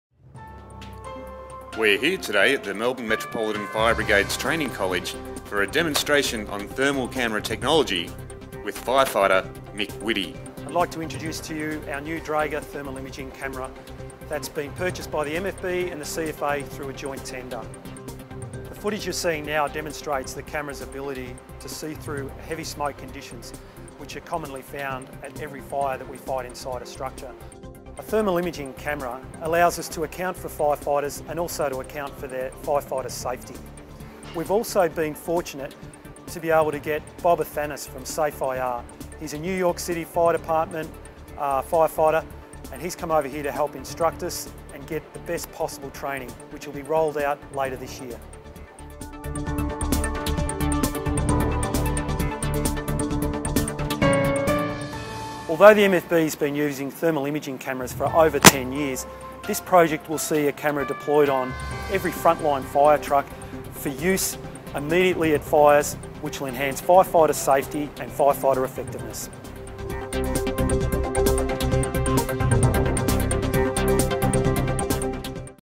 Thermal cameras for Firefighters
MFB and CFA have come together to invest in a new thermal imaging camera project which will improve the fire services capabilities at incidents.